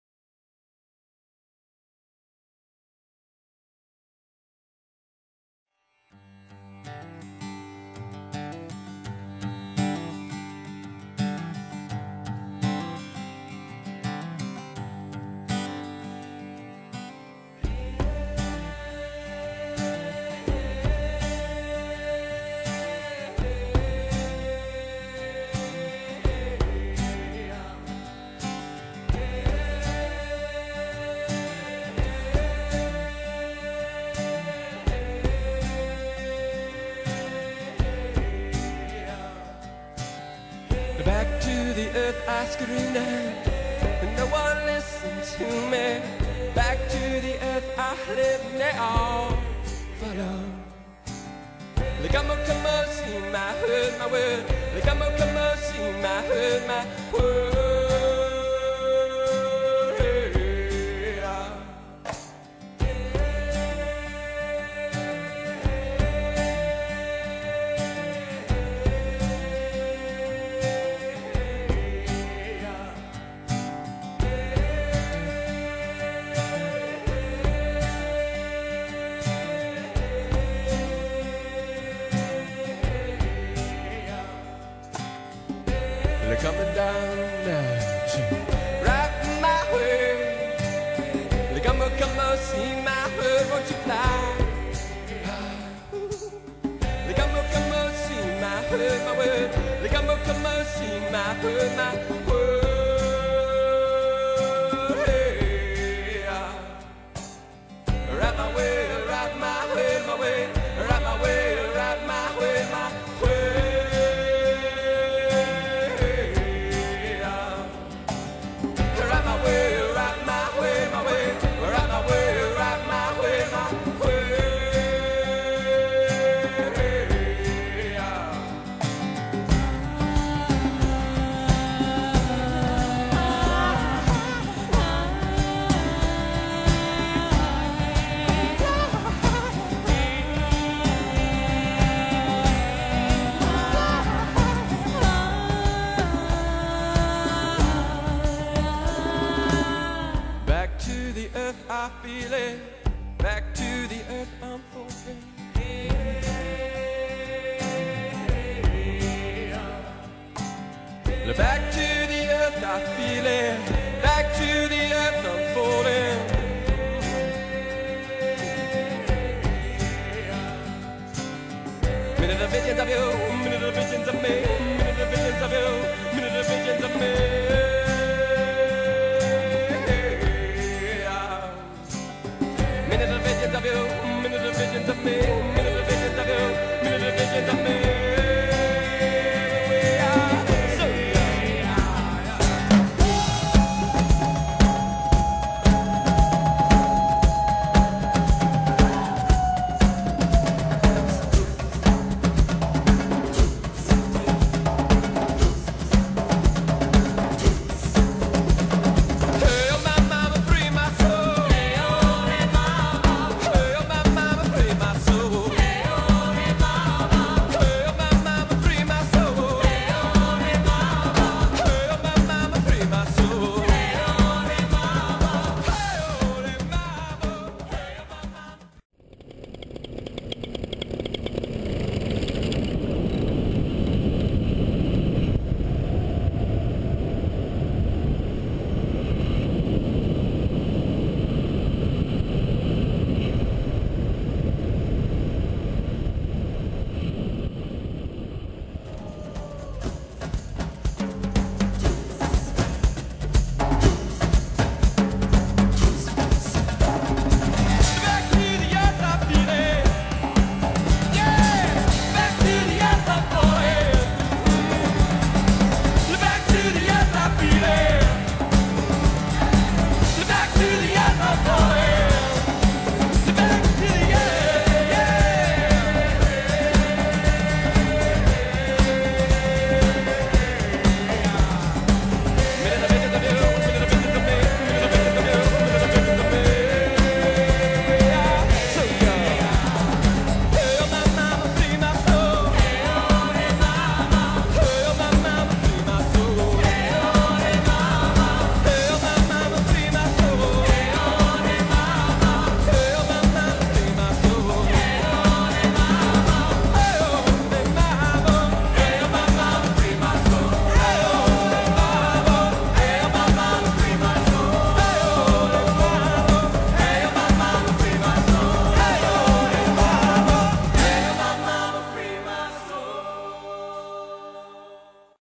Blue Ridge Beauty
Morning PPG flight with friends over the Blue Ridge mountains near Lexington, Virginia.